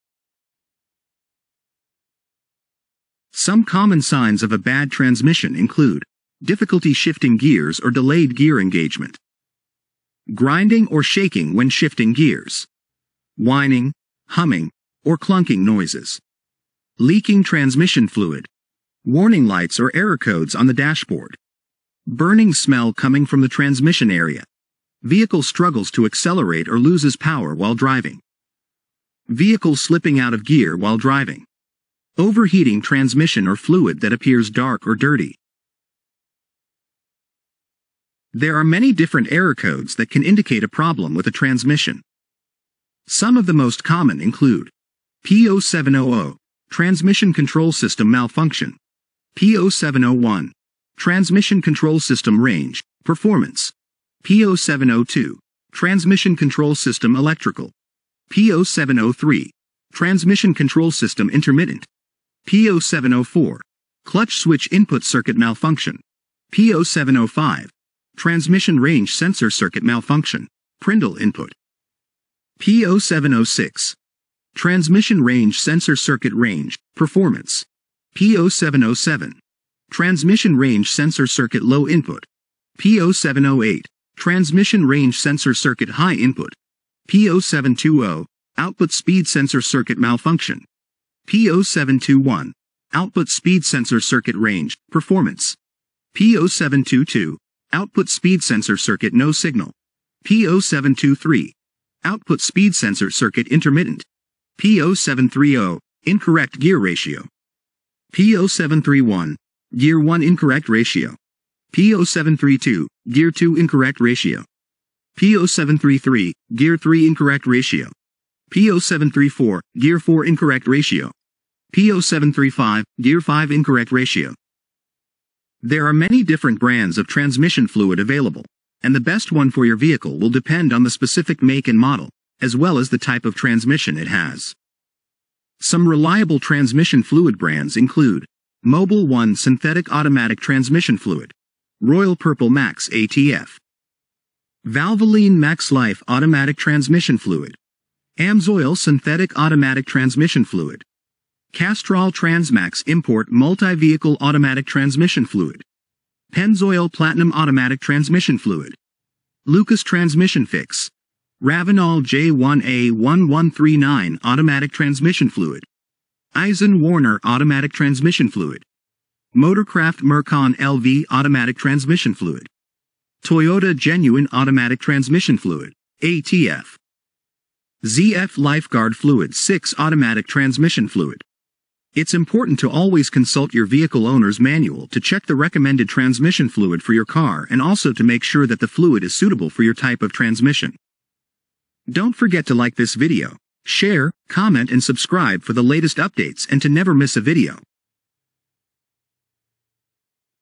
Sign of bad gearbox. Sign of bad transmission. Error codes of bad transmission.
Are you experiencing transmission slipping, delayed gear shifting, or strange noises coming from your transmission?

These could be signs of a bad transmission. In this video, we will discuss the common signs of a bad transmission and how to diagnose them.

Don't let a malfunctioning transmission cause damage to your vehicle, watch this video to learn how to identify the problem and take action.

Make sure to consult your vehicle's owner manual or a professional to ensure proper diagnosis and repair.